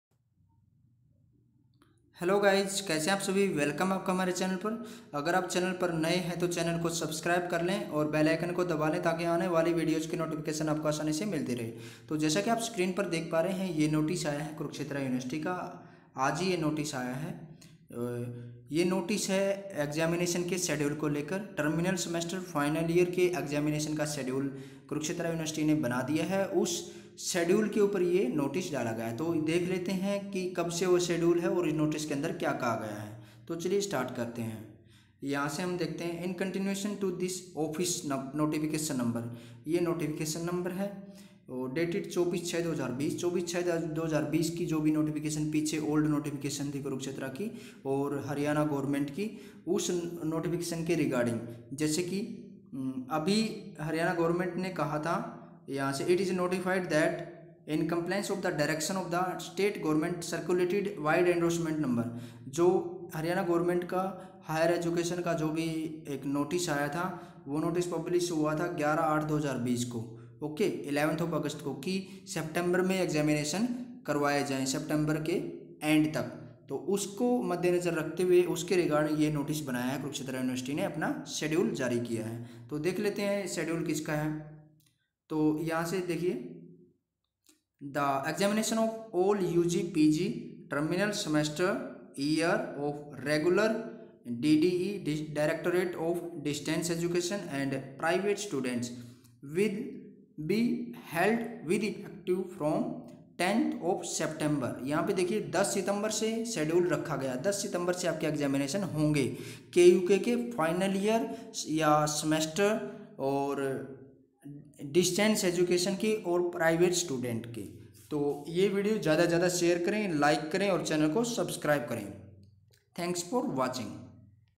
KUK exams notice, exams schedule for final year /terminal semester , dde
kuk new notice final year exams terminal semester exams
kurukshetra University